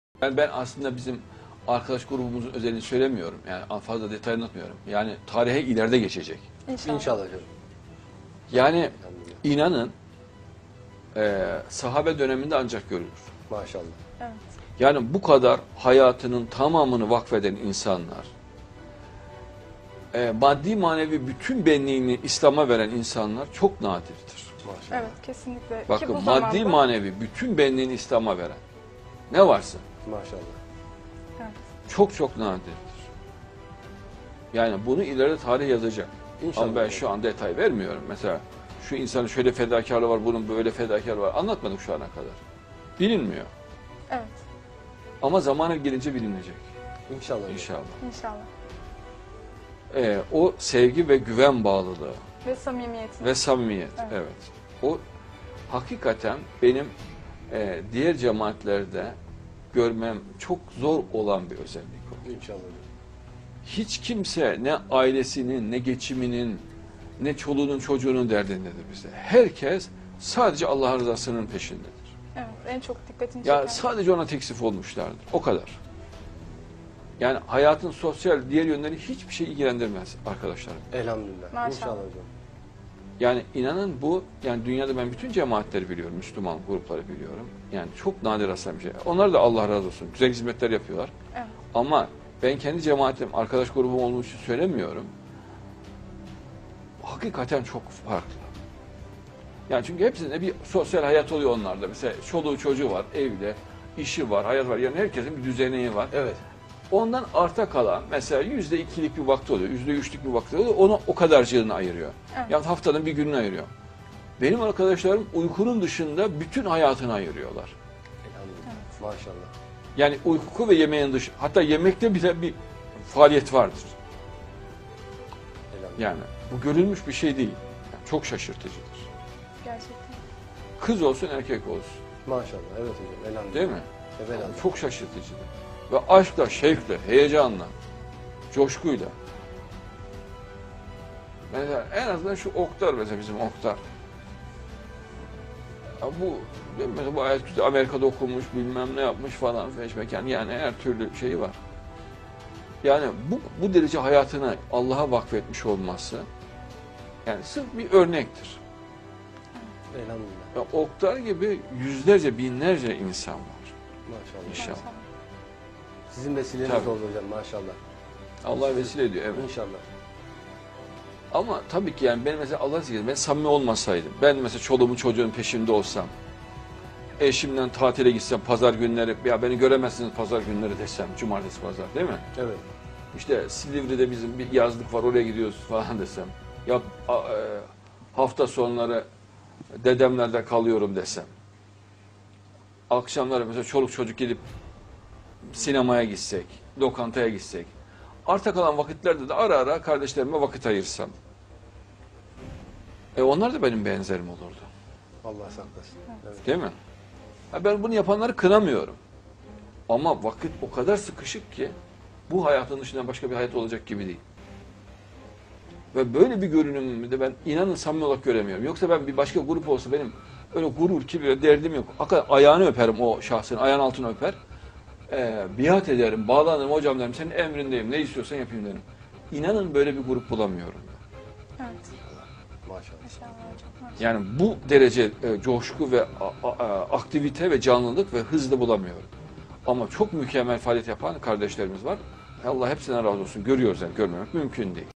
Adnan Oktar fedakar arkadaşlarını anlatıyor
Adnan Oktar: Bu kadar hayatının tamamını vakfeden insanlar, maddi manevi bütün benliğini İslam'a veren insanlar çok nadirdir. Bunu ilerde tarih yazacak inşaAllah. Herkes sadece Allah'ın rızasının peşinde.